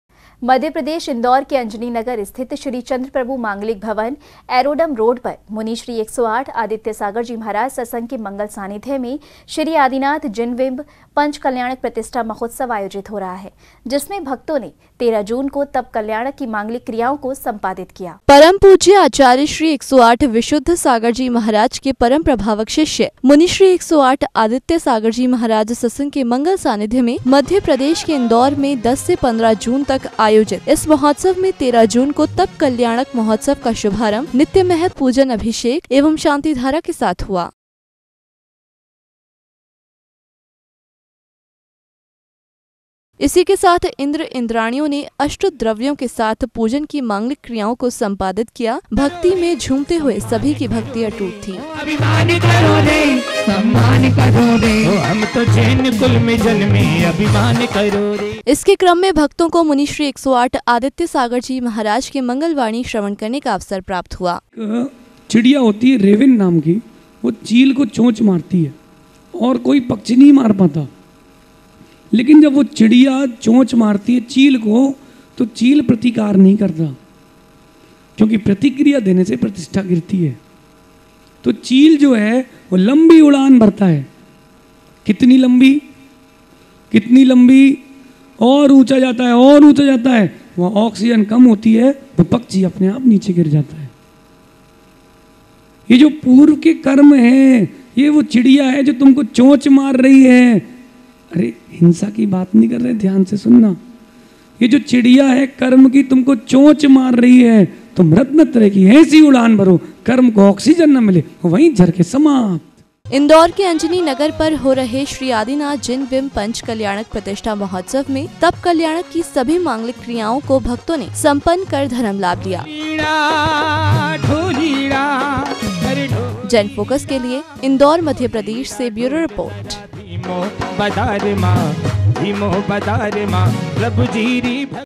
13 जून को तपकल्याणक महोत्सव मनाया गया | Anjani Nagar | Indore News | JAIN FOCUS
इंदौर के अंजनी नगर में हो रहा है पंचोत्सव
सानिध्य मुनिश्री आदित्यसागर जी महाराज ससंघ का
अंजनी नगर में देखने को मिल रही अद्भुत भक्ति